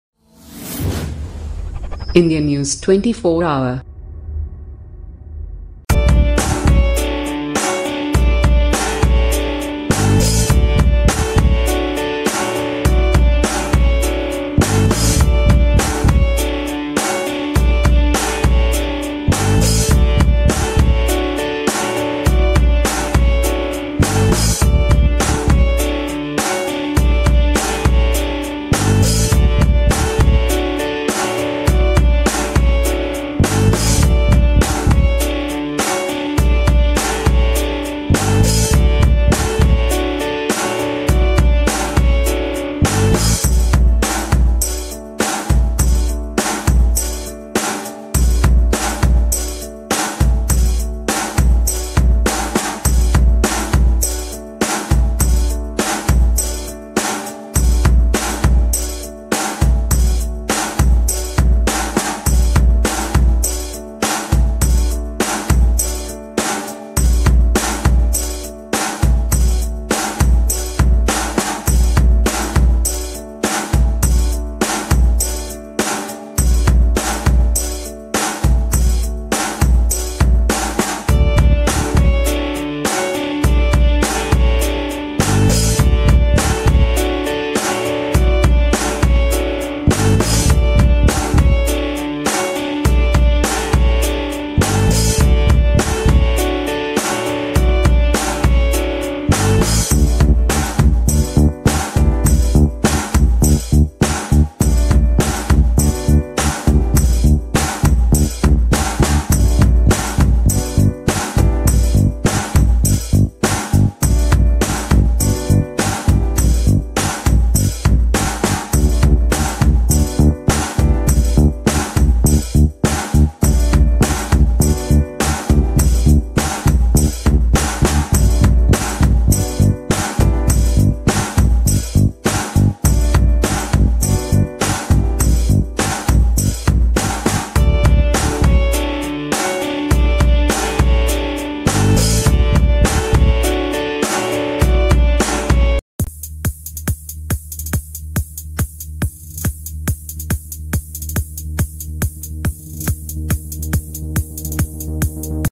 Delayed by traffic, groom takes kochi metro to his wedding
Ranjith Kumar left his car to take the Kochi Metro to get in time for his wedding in Ernakulam. Kochi:  On the biggest day of this couple's life, the Kochi Metro came to their rescue. A video posted on the Kochi Metro's official Facebook page narrate...